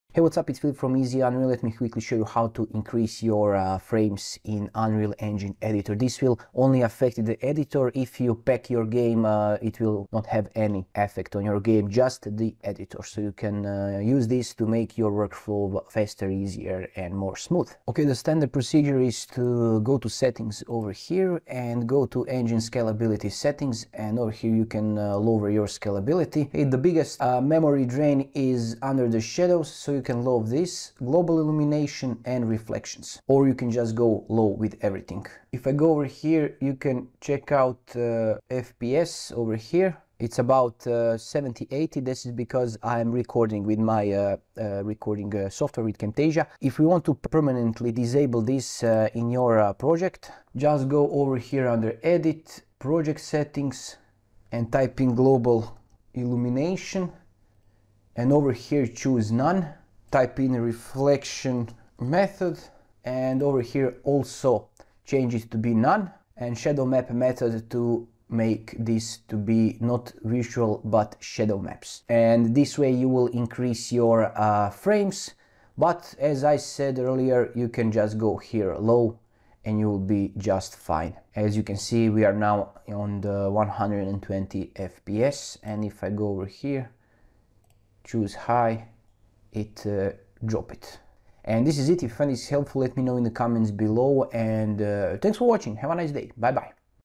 Optimize Unreal Engine 5 for Better FPS and Frame Rate in the Editor
In this video, I'll walk you through the essential tips and settings to optimize Unreal Engine 5 for better FPS and frame rate in the editor.

W / S / A / D + RMB: Move Camera
Q / E: Move Camera Up and Down
Ctrl + S: Save
Ctrl + C / Ctrl + X / Ctrl + V: Copy, Cut, Paste
Ctrl + Z / Ctrl + Y: Undo / Redo
W / E / R: Move / Rotate / Scale Objects
Ctrl + Space: Open Content Drawer
Ctrl + L: Change Direction f The Sun
F11: Toggle Full Screen